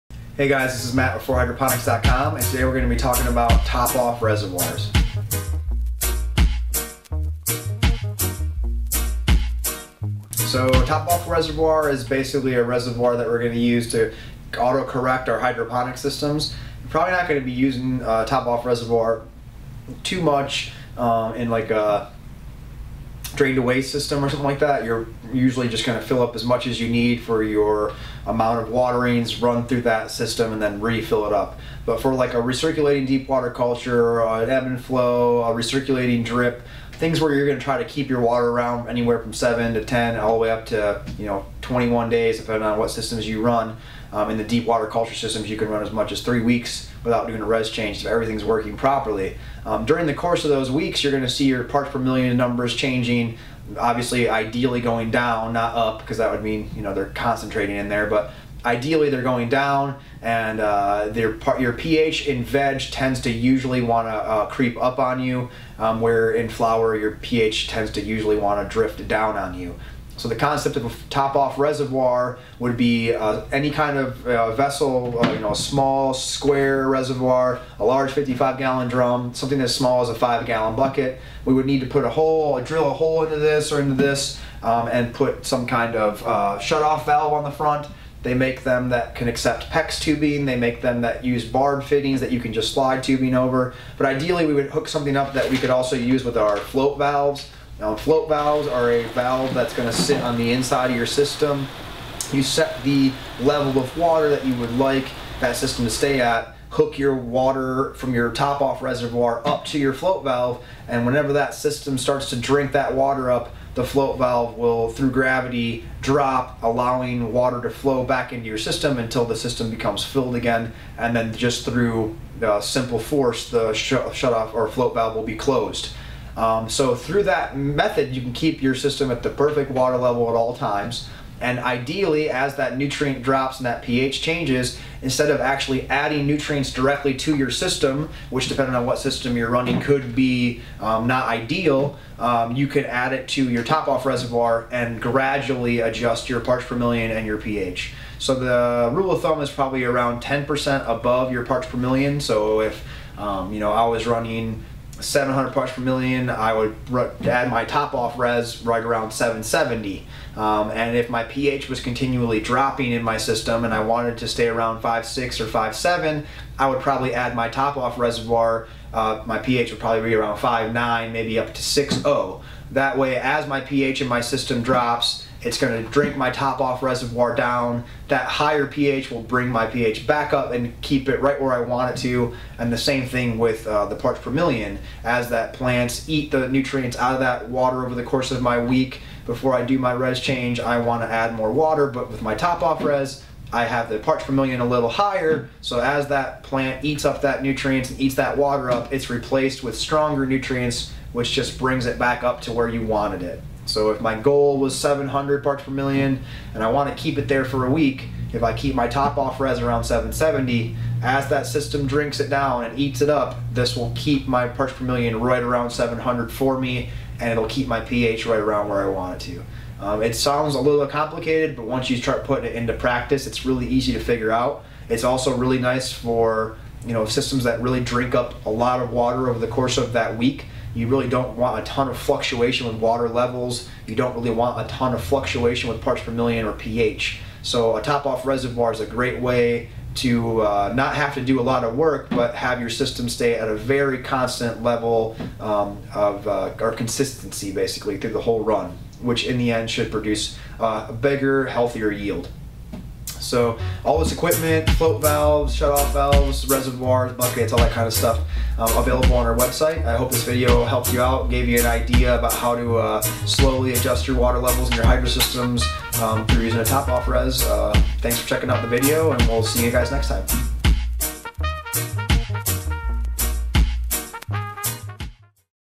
How to use a Hydroponic Back Up Reservoir
If building a Hydroponic system that needs to be "topped off" to maintain proper pH and PPM a back up reservoir could save you time and be a more stable option.

A back up reservoir keeps your system when you want it by automatically maintaining your water level and re-filling it with h2o or a solution of your choosing.

Matt will explain a few tools and techniques used in adding a Back Up Reservoir to your system in this video and remember you can get all of the supplies seen here and more